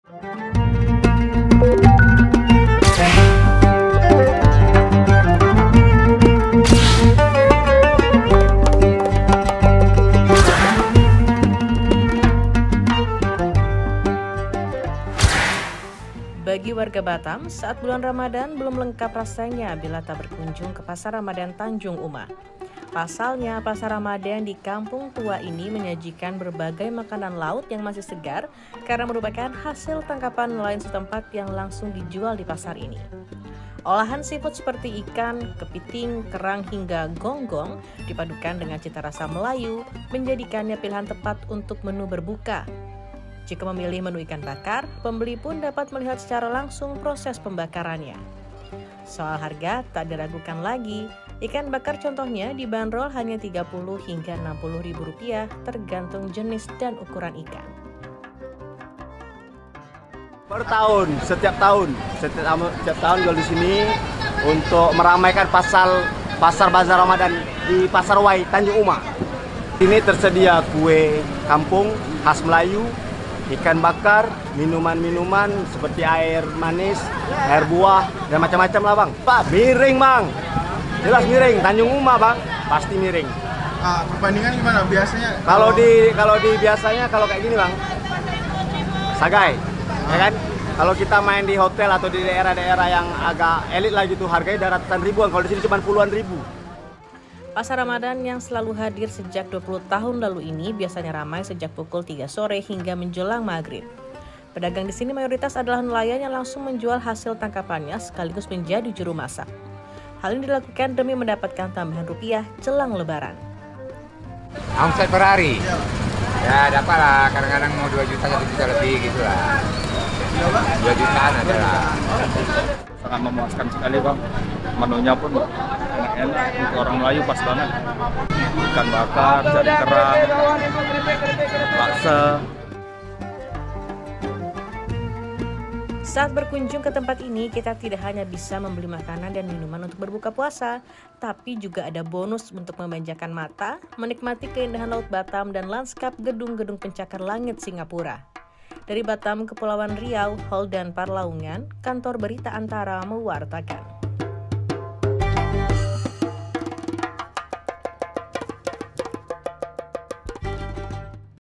Ngabuburit berbelanja ikan segar di Pasar Ramadhan Kampung Tua Batam
ANTARA - Pasar Ramadhan Tanjung Uma yang ada di Kecamatan Lubuk Baja, Batam, Kepulauan Riau menyajikan berbagai makanan khas Melayu dan olahan makanan laut. Selalu hadir dalam momentum tahunan sejak 20 tahun lalu, pasar ini ramai dikunjungi pembeli menjelang berbuka puasa karena harga yang terjangkau. (Holdan Parlaungan/Rayyan/Amita Putri Caesaria)